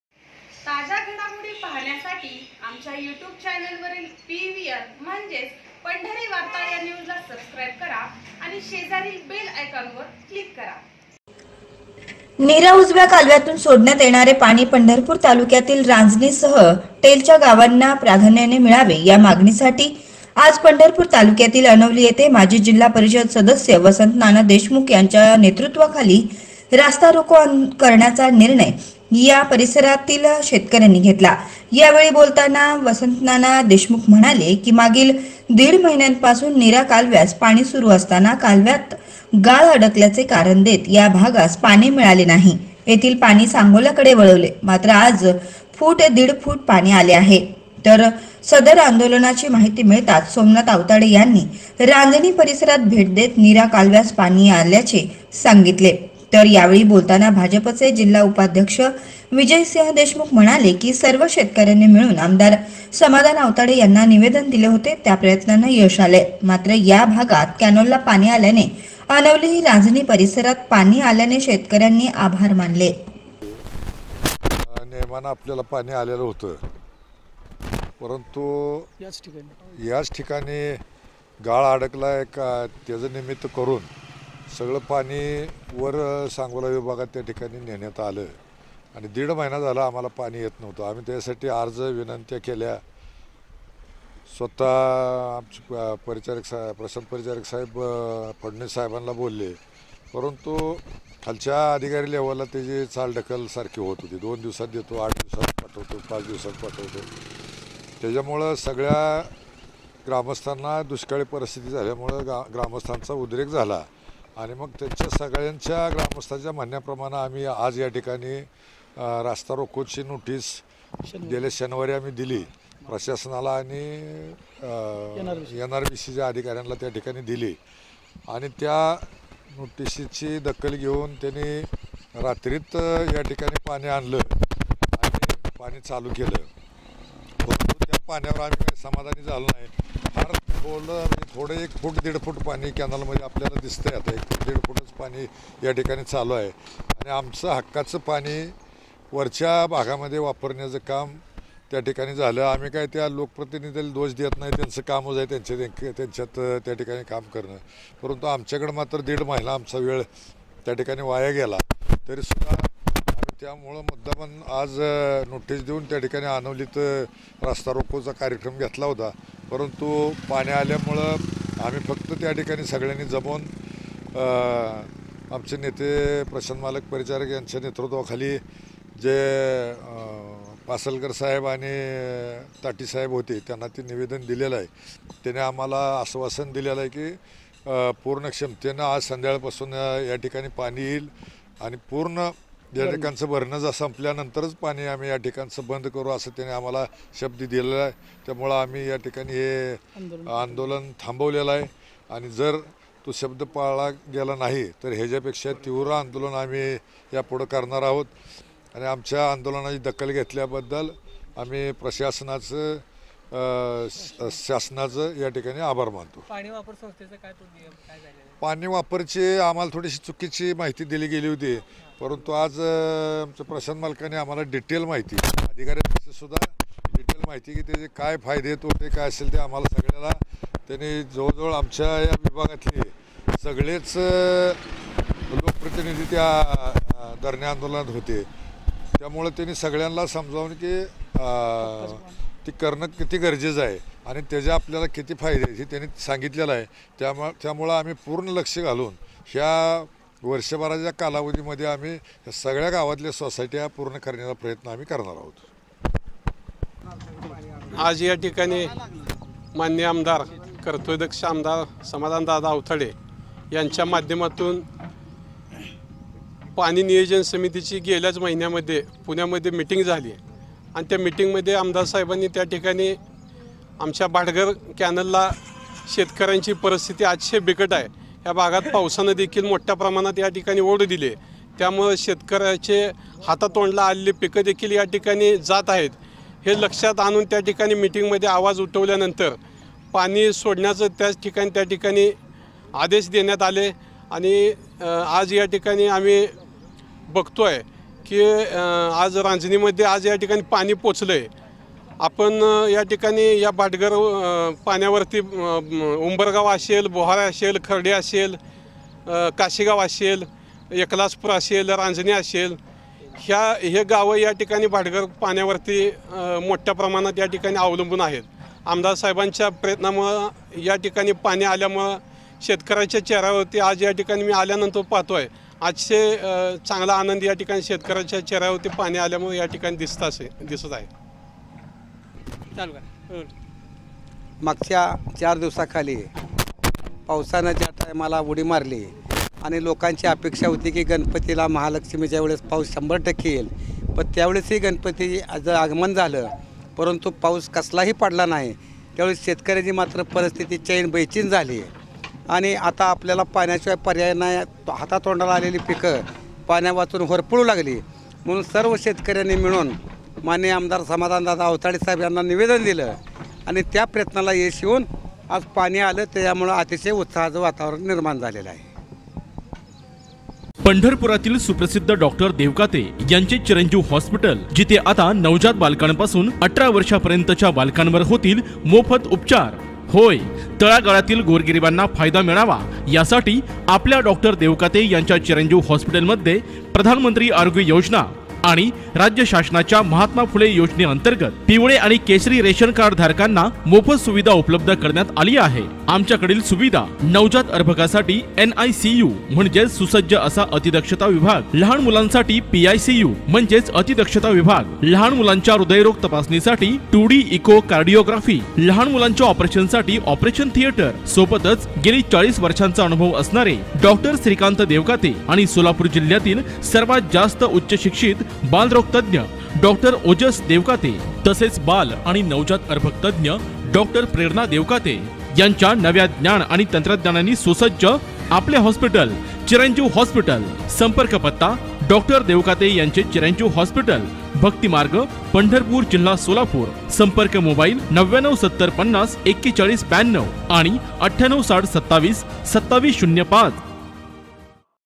अनवली चौकात रस्ता रोकोचा इशारा अन कालव्यास आले पाणी
इशाऱ्यानंतर फूट दीडफूट पाणी आले - वसंतनाना देशमुख
आ.आवताडेंच्या पाठपुराव्याने रांझणीत पाणी पोहोचले - सोमनाथ आवताडे